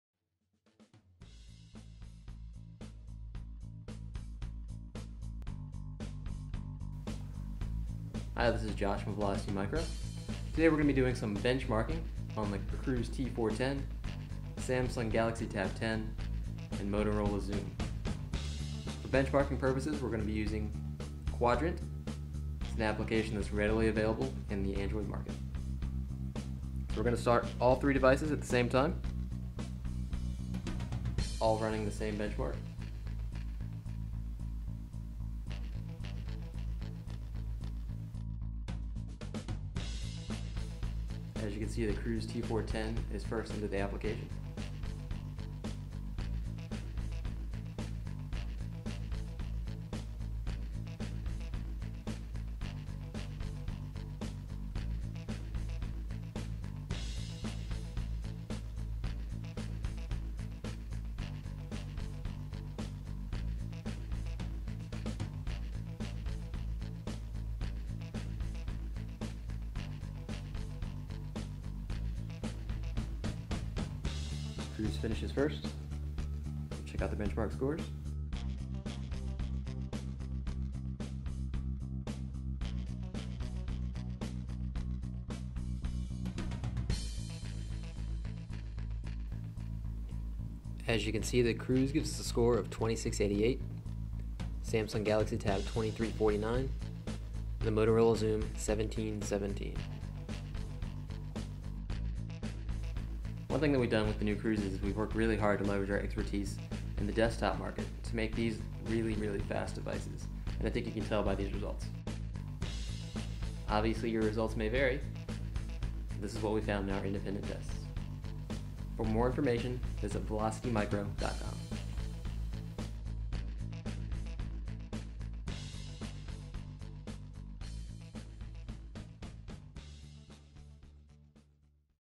Cruz T410 Quadrant Benchmark
We benchmarked the Cruz T410 using Quadrant to see how it compared to some of the higher priced competition. The results are indicative of the ultra fast performance the T410 offers at a fraction of the price.

Your results may vary.